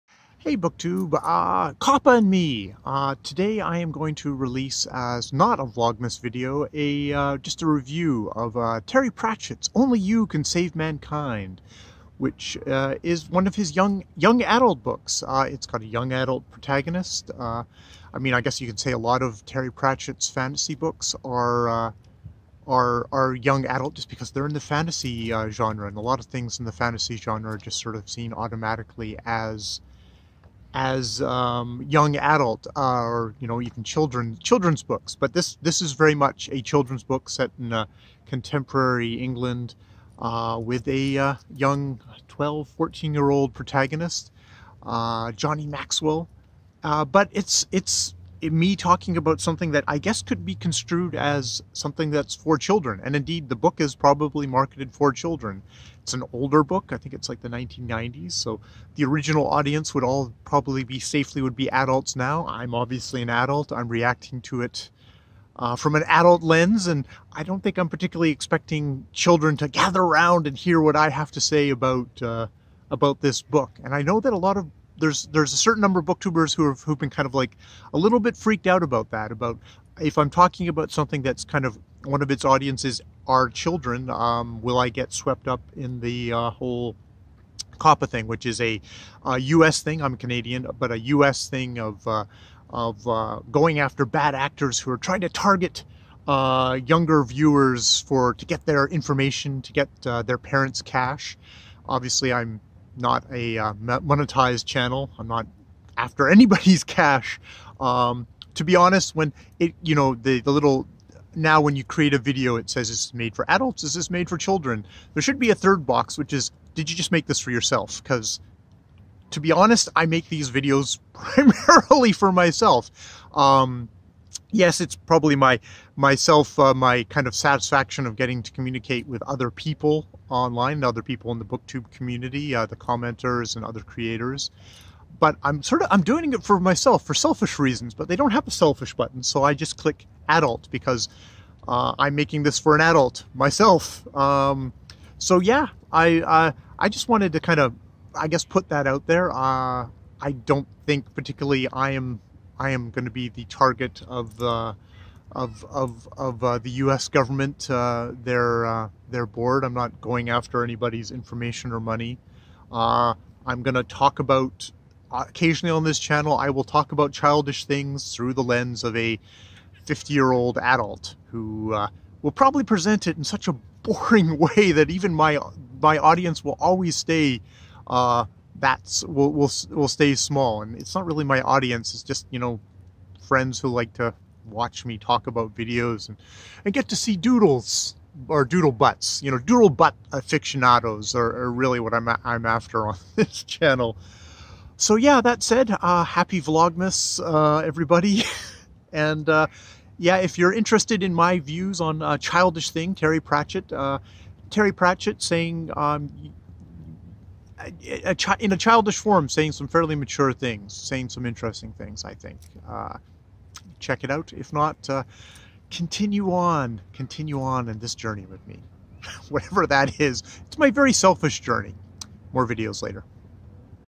Vlogmas Day 15 - COPPA and Me, A Selfish Video
My low energy version vlogmas, or a 24 day therapy session to get me through the Christmas month - or at least to December the 24th. So the goal (for me) is to put out one short video each day up to Christmas Eve.

(Video #261)